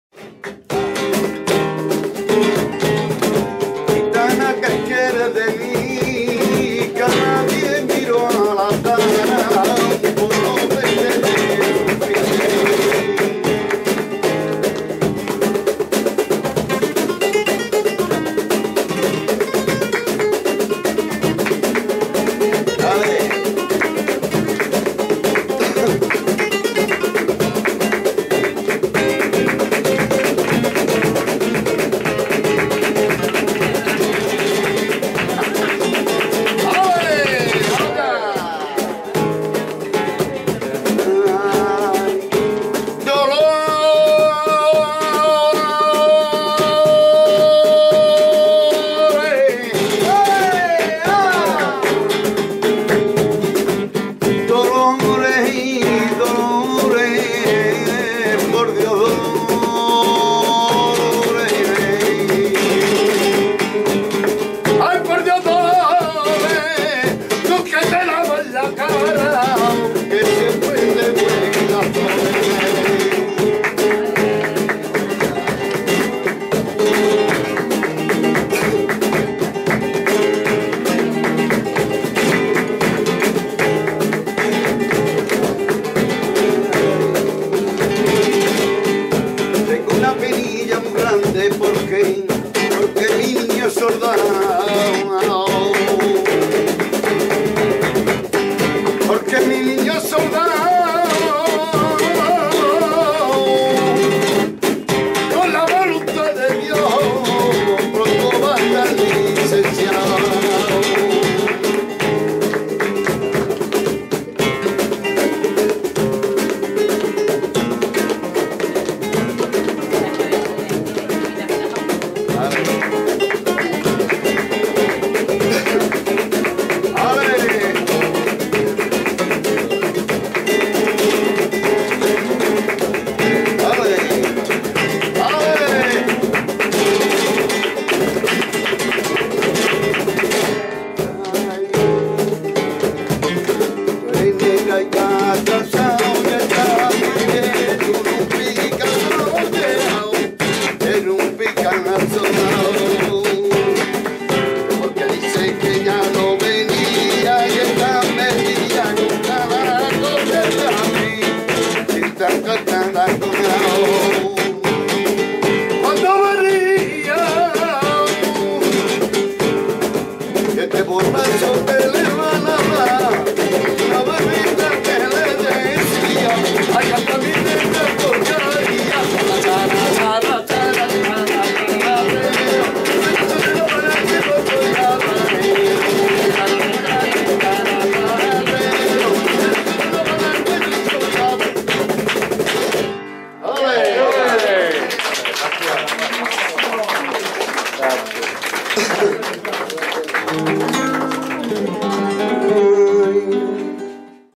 Flamenco Granada cavern - 21st April 2018 - Part 2
The barrio Sacromonte s said to be the home of flamenco. It is just up the Darro river not far from the Alhambra in Granada, Spain.  The district is known for its caverns dug out of the soft earth of the hillside. By the 16th century there was a large gypsy population living there. The music speaks for itself...For part 1 and for historical footage of the area